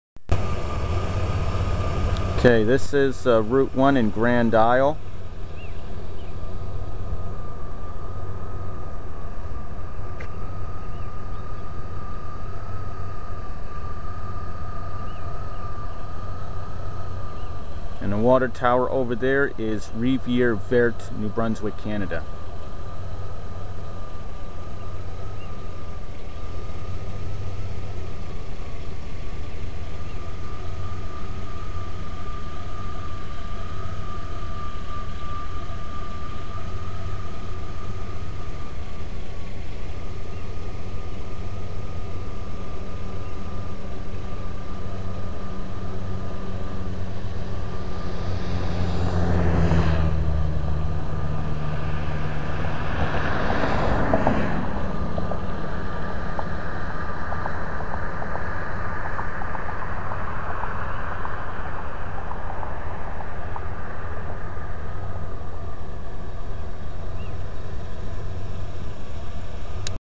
A view of Rivière-Verte, N.B., Canada; from Grand Isle, Maine.
Joseph Normand Grinnell make a small movie of the view of Rivière-Verte, New Brunswick, Canada; as seen from Grand Isle, Maine, United States; on July 5, 2010.